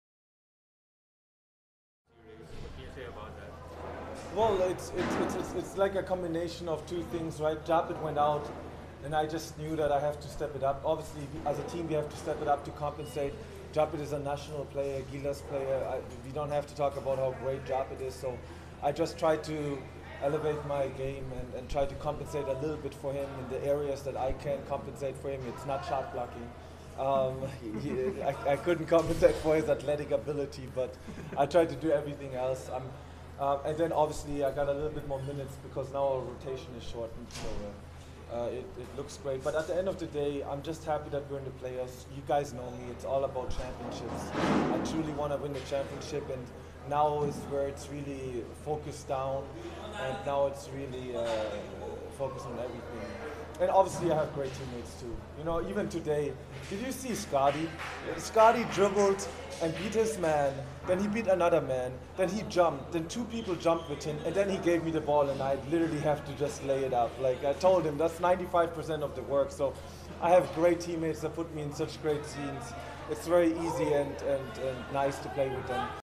Christian Standhardinger on being in contention for a Best Player of the Conference award | PBA
Barangay Ginebra big man Christian Standhardinger on being in contention for a Best Player of the Conference award